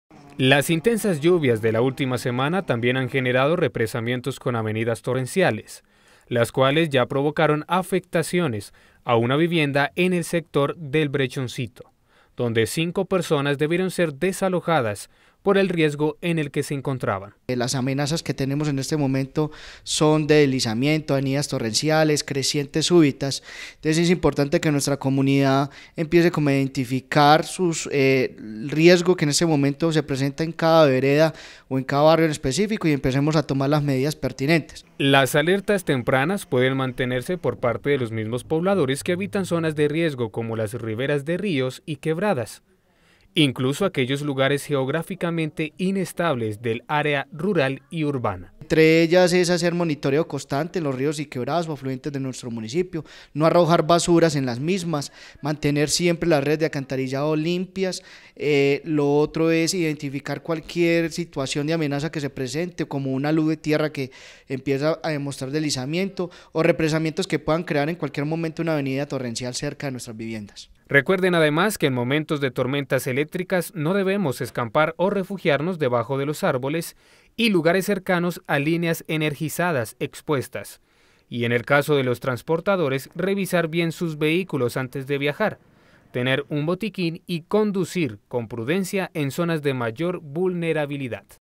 Alertas y recomendaciones para temporada invernal
Las autoridades, organismos de gestión del riesgo generan algunas
alertas y recomendaciones, debido a las emergencias causadas por el invierno